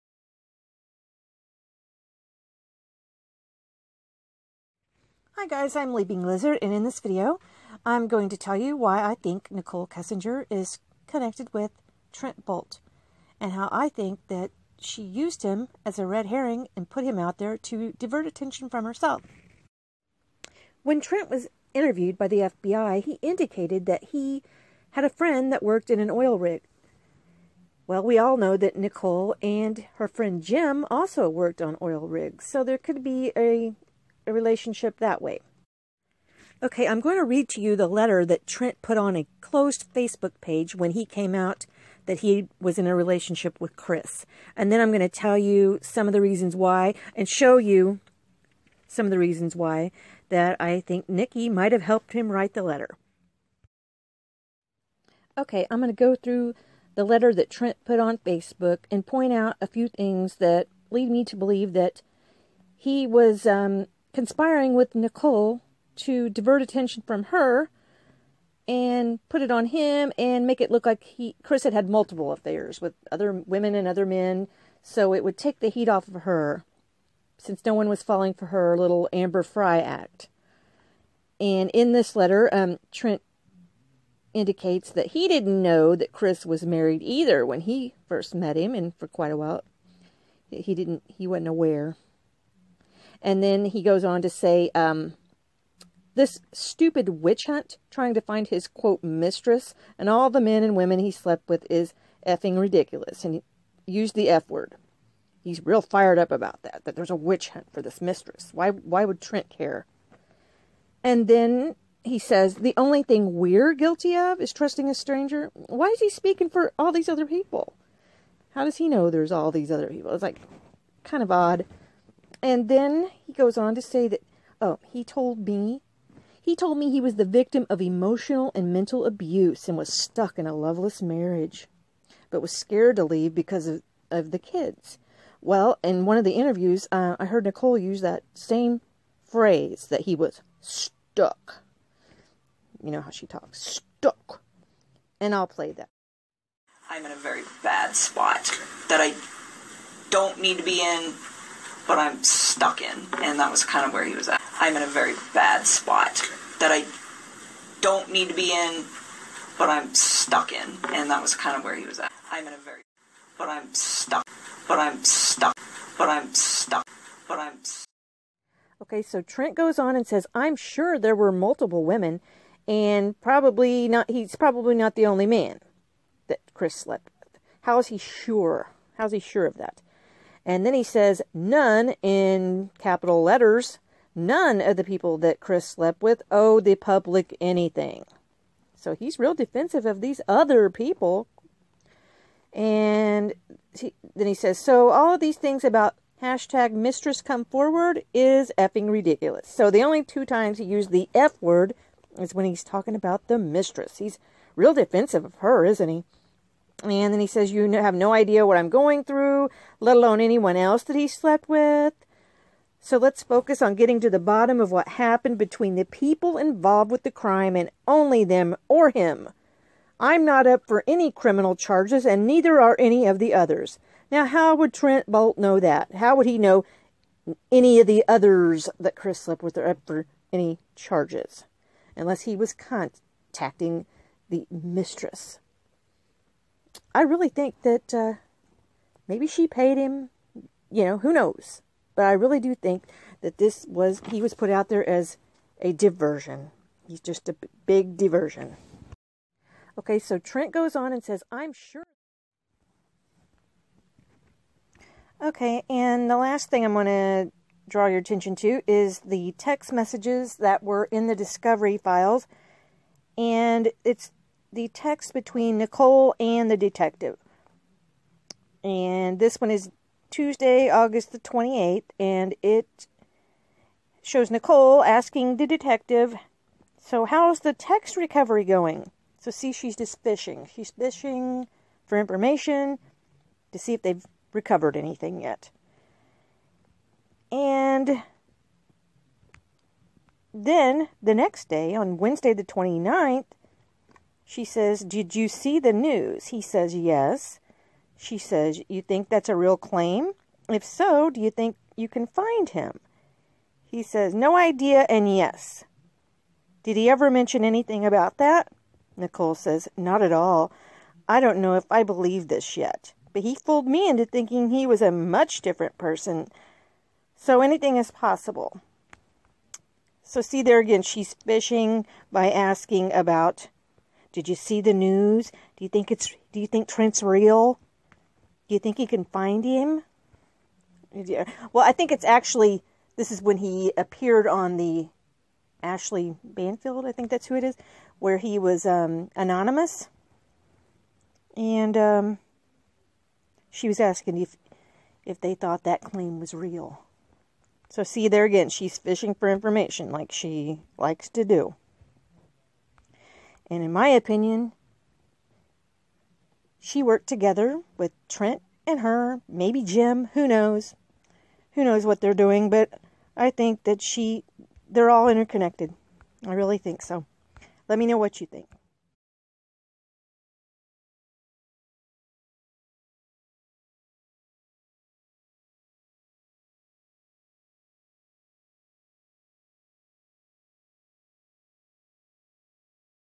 Did Nichol Kessinger Chris Watts Mistress Conspire with Trent Bolte?
My commentary about Trent Bolte and a possible connection with Nikki Kessinger

EDIT UPDATE
These were my feelings at the time I made this video, my feelings have since changed and I no longer feel Trent was involved, Nichol K, the CBI has stated they are convinced she played NO Part